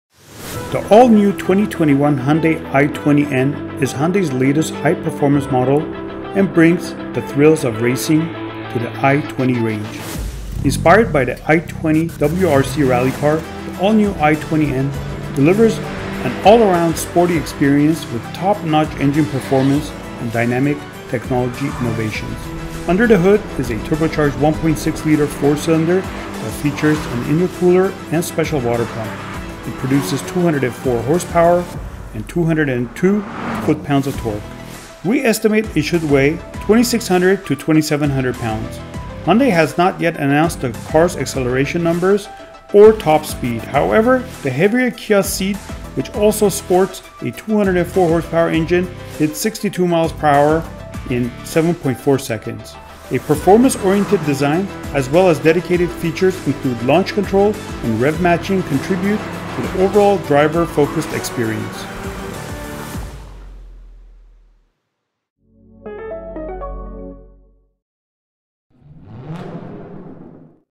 2021 Hyundai i20 N Highlights
The all-new 2021 Hyundai i20 N is Hyundai’s latest high-performance model and brings the thrills of racing to the i20 range.

Inspired by the i20 WRC rally car, the all-new i20 N delivers an all-around sporty experience with top-notch engine performance and dynamic technology innovations.

Under the hood is a turbocharged 1.6-liter four-cylinder that features an intercooler and special water pump. It produces 204 horsepower  and 202 pound-feet of torque. We calculated high, believing the i20 N should weigh around 2,645 pounds (1,200 kilograms). Hyundai has not yet announced the car's acceleration numbers or its top speed. However, the heavier Kia Ceed, which also sports a 204-hp engine, hits 62 miles per hour takes 7.4 seconds. A Ford Fiesta ST needs 6.5 seconds.

A performance-oriented design as well as dedicated features including Launch Control and Rev Matching contribute to an overall driver-focused experience.